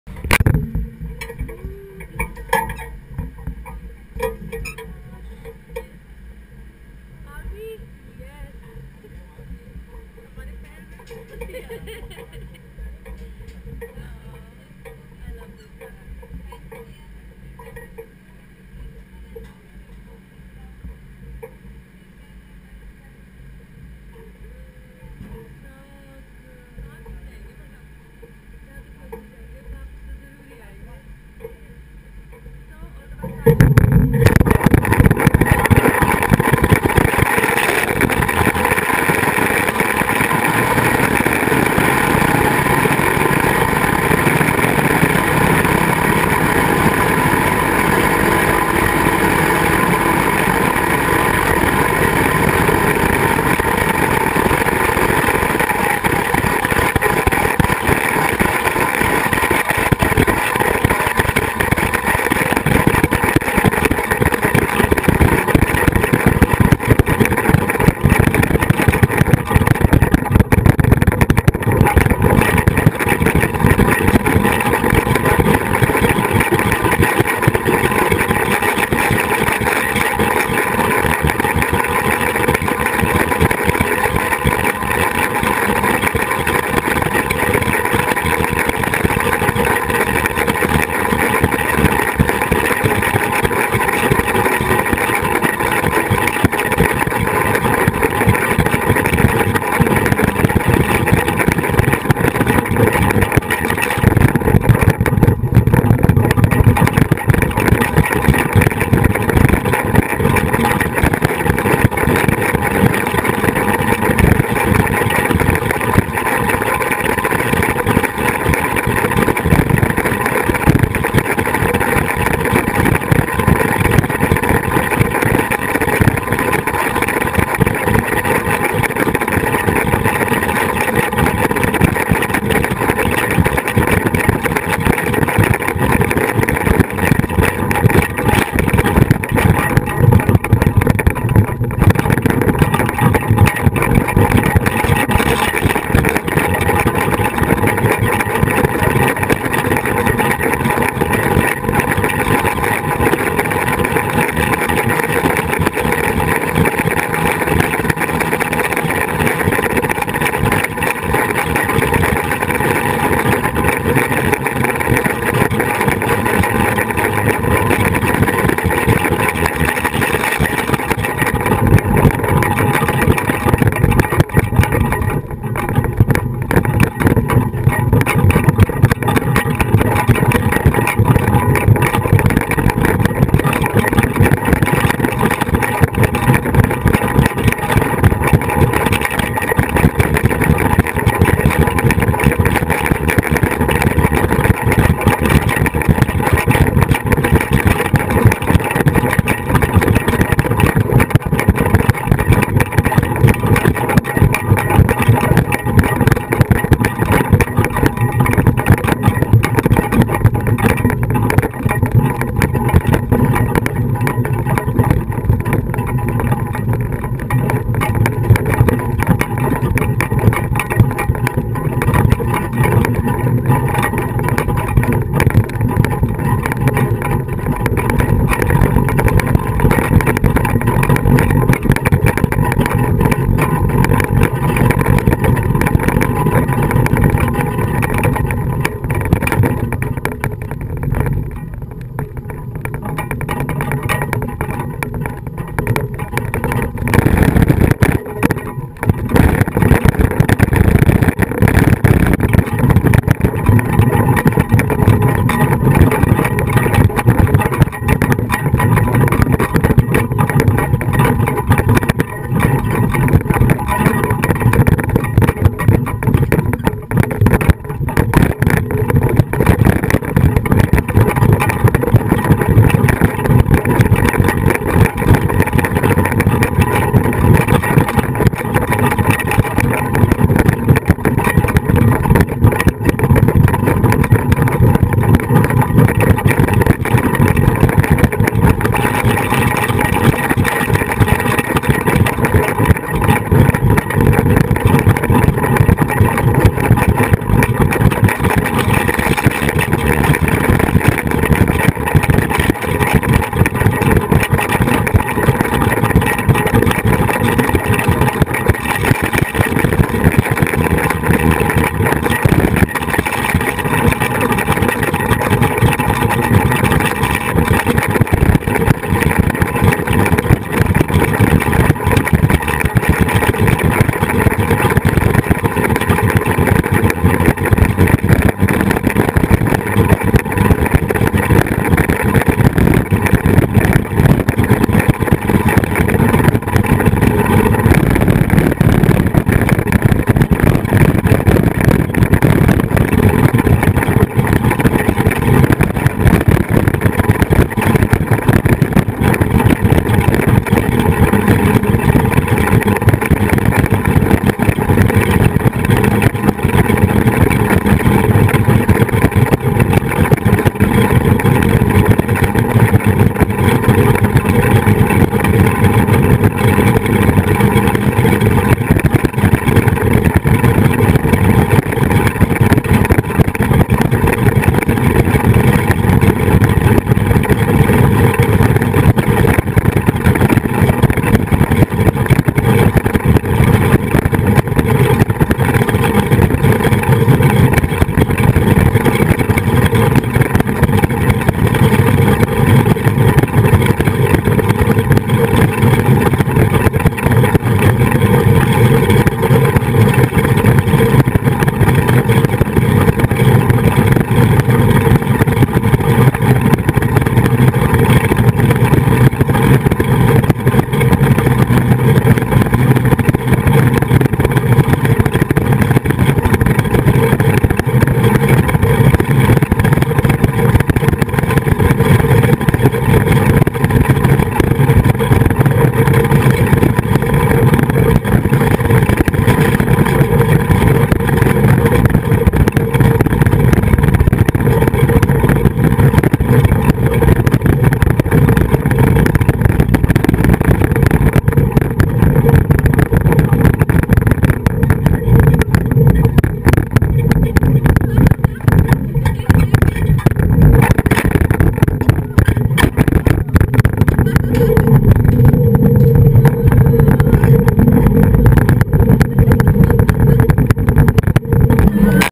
FLYING FOX in RISHIKESH had a great experience and wonderful weekend
RISHIKESH, flying fox, adventure , life experience .